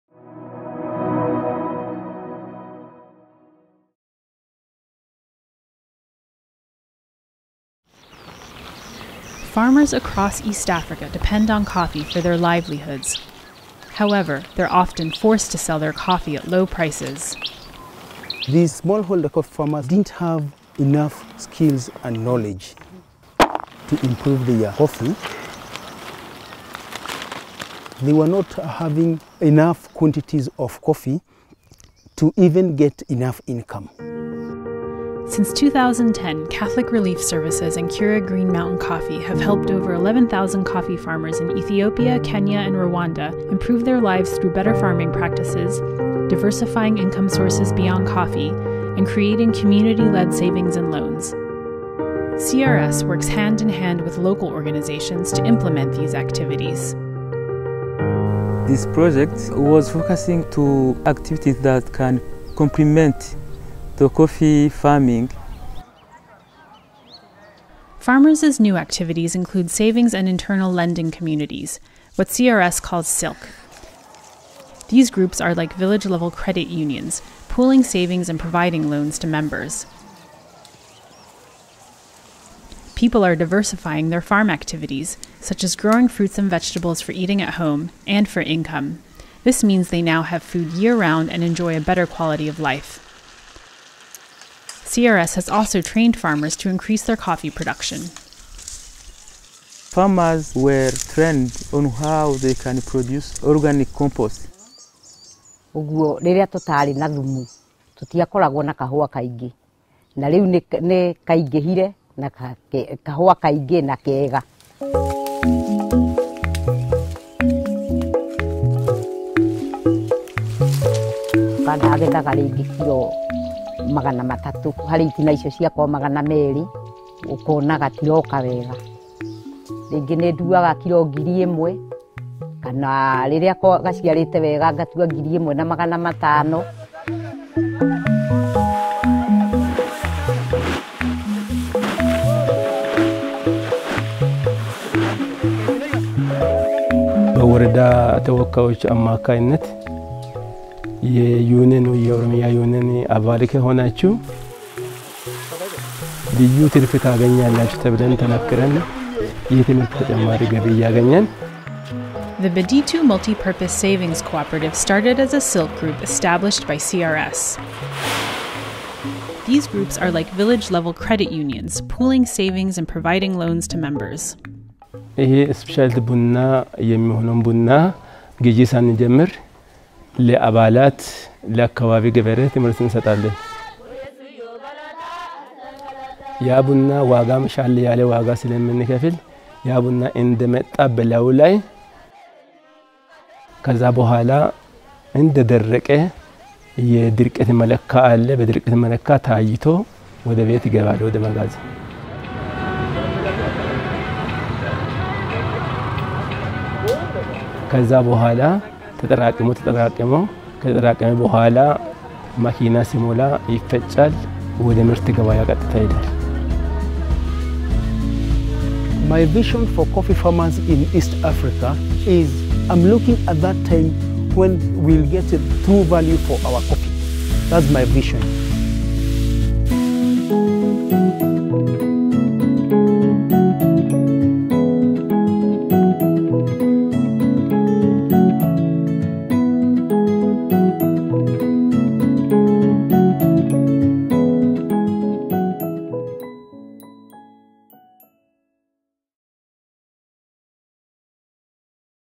Fair Trade Brings New Perks to Coffee’s Homeland
In partnership with Keurig Green Mountain, CRS is helping traditional coffee farms in Ethiopia, Kenya and Rwanda to improve their coffee quality so farmers can access more profitable international fair trade and specialty coffee markets. Without the know-how, farmers couldn’t get a good price for their coffee, which years ago used to pay their way almost entirely. But prices since plunged. Now farmers produce a high-quality niche product, and have diversified their farms so they have food and income all year round.